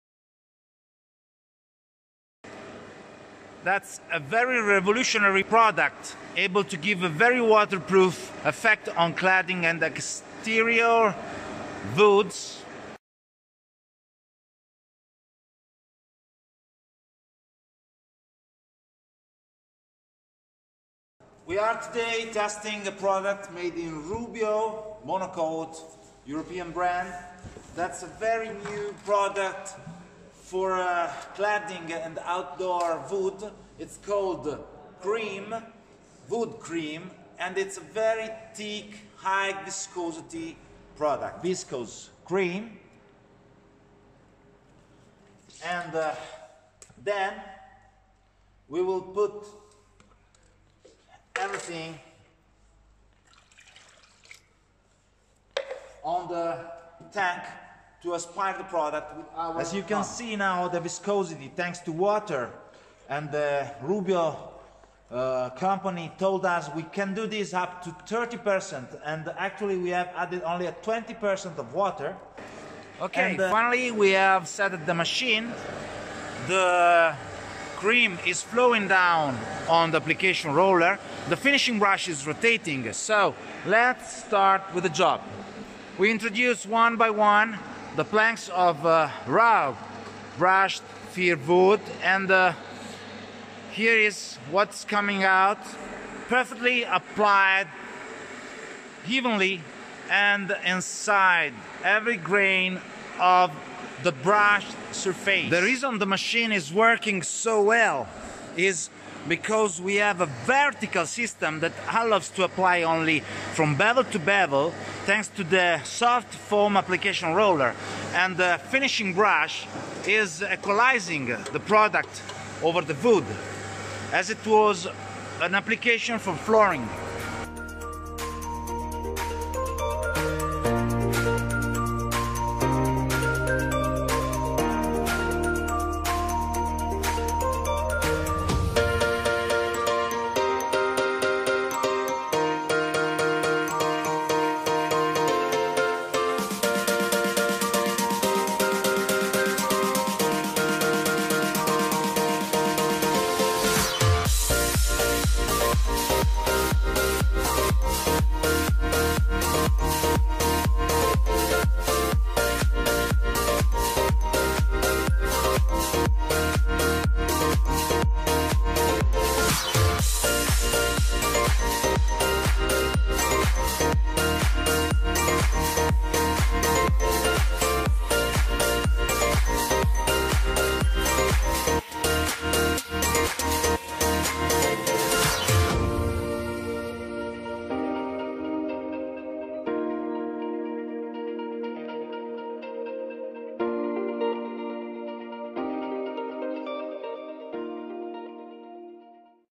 Rubio Monocoat Wood Cream Bolt Black application with machine Paoloni Minitunnel
on that video Paoloni company team shows how easy it is to apply an high viscosity cramy product made in Rubio Monocoat brand. that's a water proof water based product so tick that manual application is very hard, so we had the suggestion by Rubio Australian distributor to test the Wood Cream with 20% of water, the product still get its quality and capability of protection and water proof resistance.
In that test we used the most high viscosity product, the Bolt Black Wood Cream color. the wood is soft Sweden fir wood wire brushed cladding 135mm wide and 18mm tick.

Themachine at work is Minitunnel320, unit of Paoloi production that you can see on our web

or call (also whatsapp available) to: ++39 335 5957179